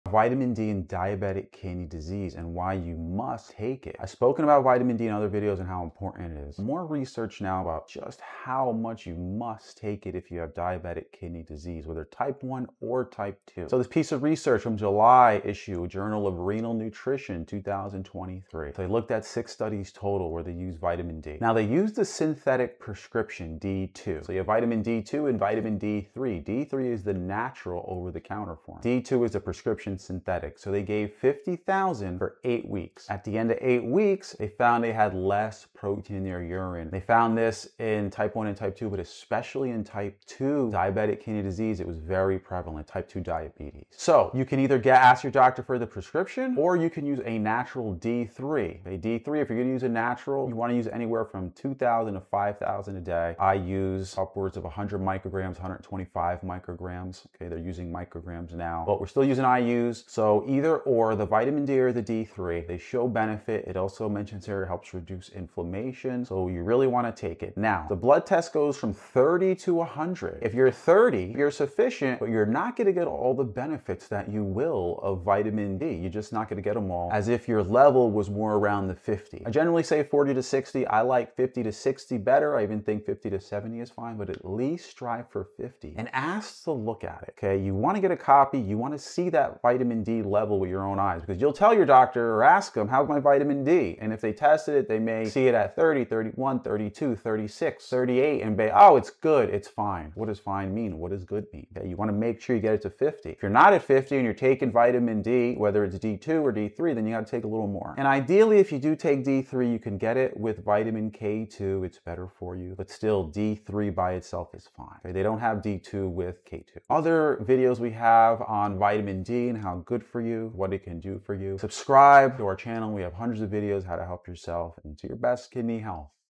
Diabetic Kidney Disease And Vitamin D
Welcome to @HealthyKidneyInc! I'm Robert Galarowicz, I've been through every stage of kidney disease, I've been living with a Kidney Transplant for 20 years, and I'm a naturopath and nutritionist.

Diabetic Kidney Disease and Vitamin D: What’s the Link?

A) Diabetic Kidney Disease (DKD) is one of the most common complications of diabetes, and managing it involves more than just blood sugar control. Research shows that Vitamin D plays an important role in kidney health, especially for people with diabetes. Vitamin D isn’t just for bone strength—it also helps regulate blood pressure, reduce inflammation, and support the immune system. In patients with DKD, Vitamin D levels are often low, and this deficiency may worsen kidney damage by increasing protein loss in the urine (proteinuria) and accelerating the progression of kidney disease. That’s because Vitamin D helps suppress the renin-angiotensin system (RAS), which, when overactive, can raise blood pressure and strain the kidneys.

Should You Supplement?

A) For many with DKD, Vitamin D supplementation may be recommended, especially if blood tests show a deficiency. Some doctors may prescribe active forms of Vitamin D (like calcitriol) rather than over-the-counter forms, especially in more advanced kidney disease, since the kidneys lose the ability to convert Vitamin D into its active form. However, it’s important to work with a healthcare provider, since taking too much Vitamin D can lead to high calcium levels and other complications, especially in people with reduced kidney function.

To learn more on how to support good kidney health, check out our